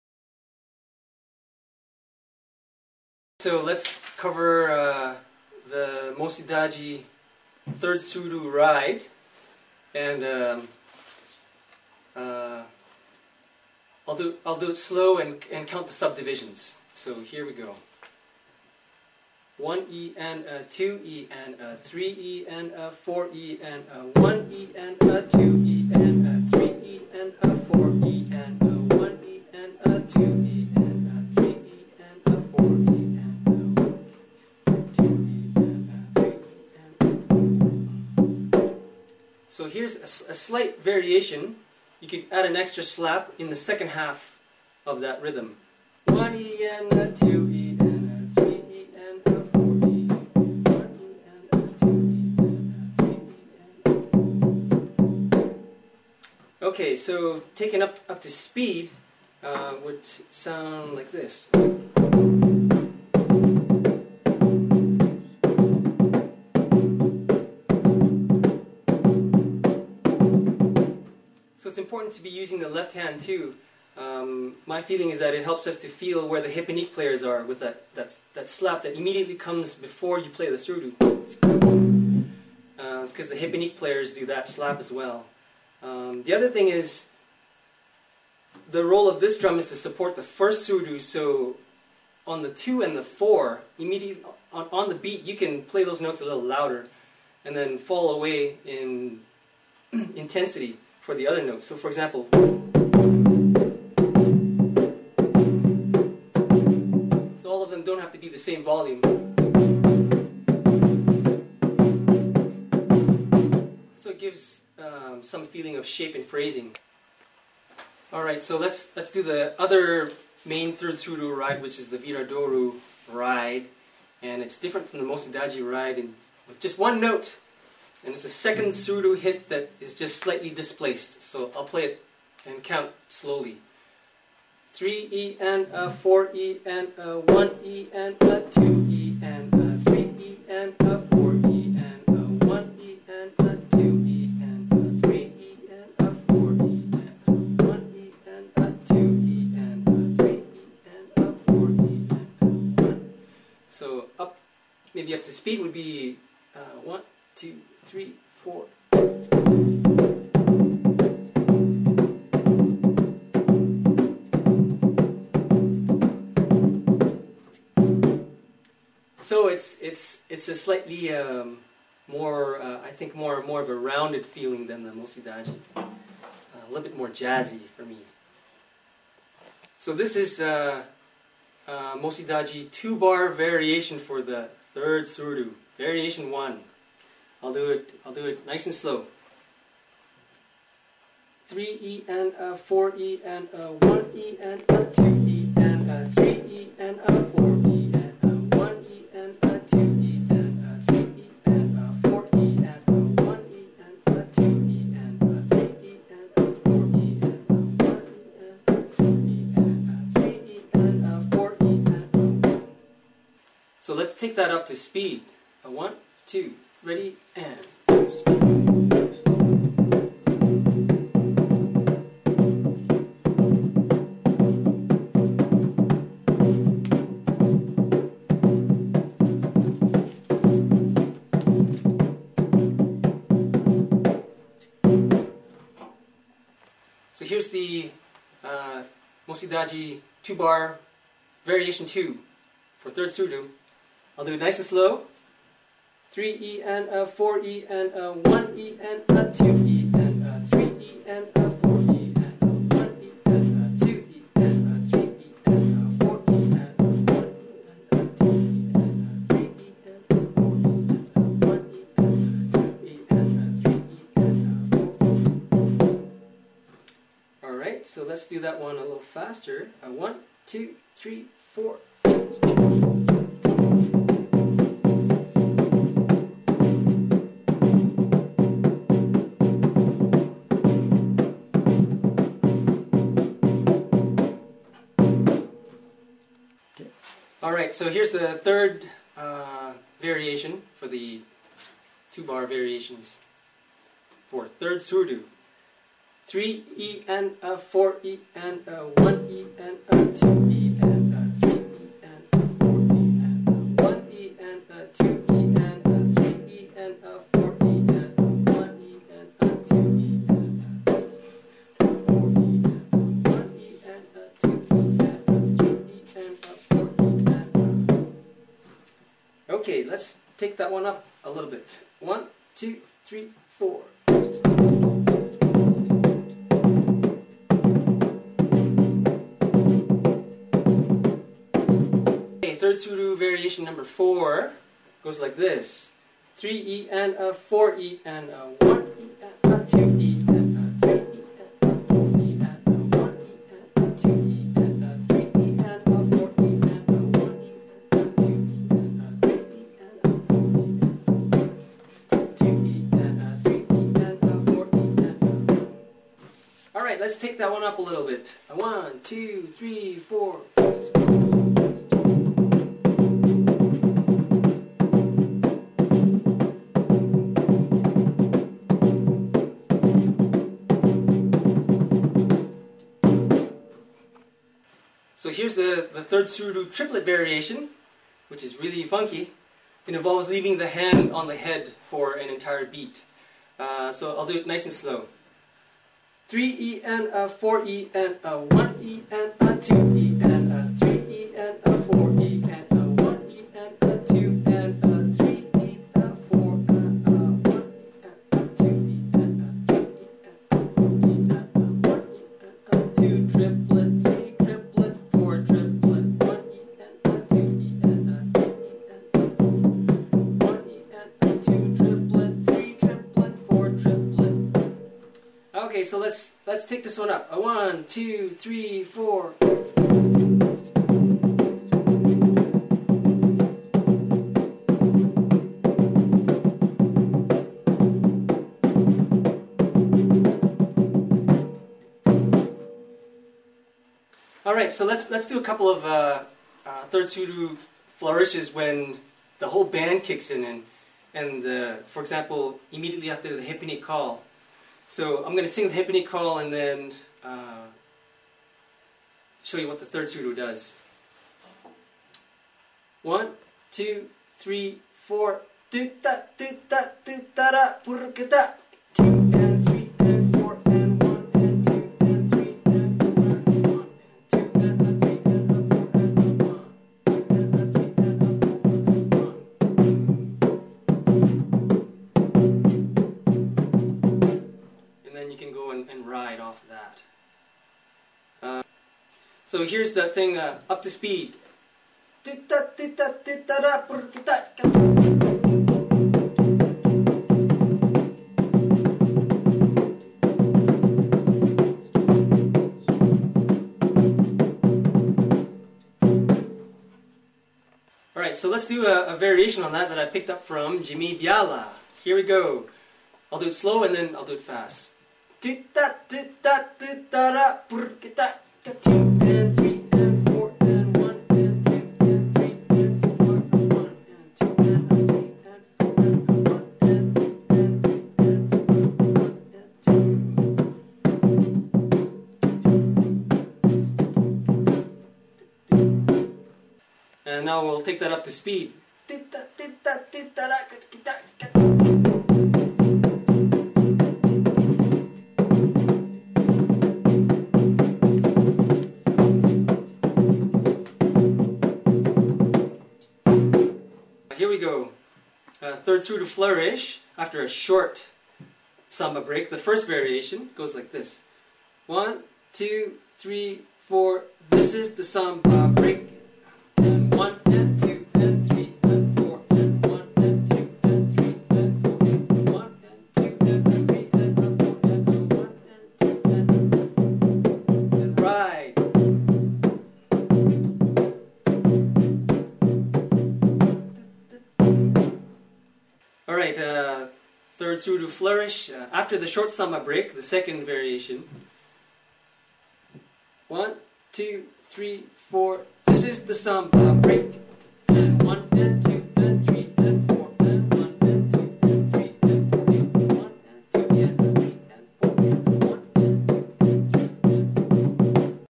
Rio Samba Surdo3 Line Library
Calgary School of Samba director Malcolm Lim demonstrates the surdo 3 lines used in Rio Samba